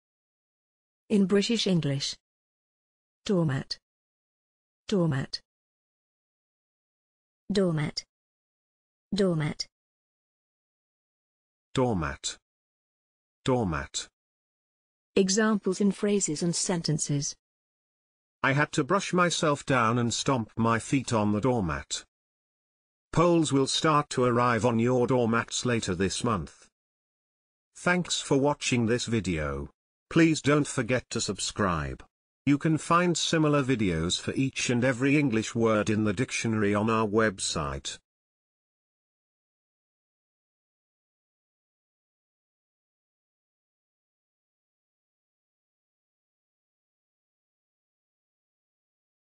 doormat - pronunciation + Examples in sentences and phrases
This is the pronunciation of doormat in British English + a few examples of its pronunciation in phrases and sentences.